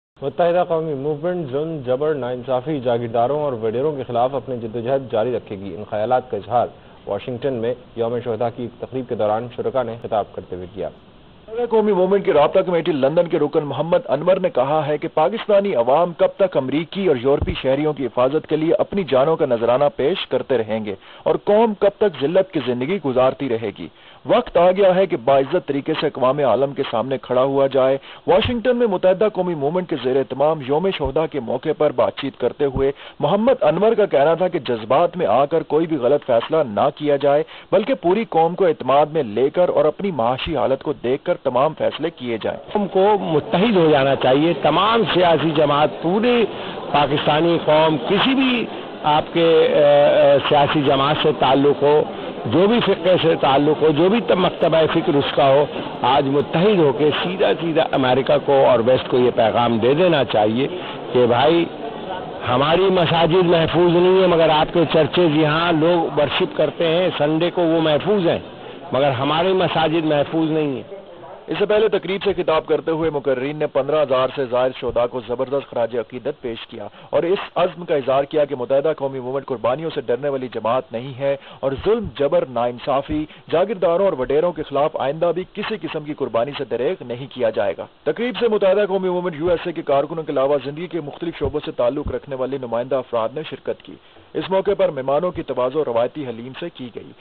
MQM Leader M. Anwar address gathering to remember "Martyrs of Revolution" in Washington
Member of MQM's Central Coordination Committee London  M. Anwar addresses a gathering in Washington DC, that was organized by MQM USA to remember "Martyrs of Revolution".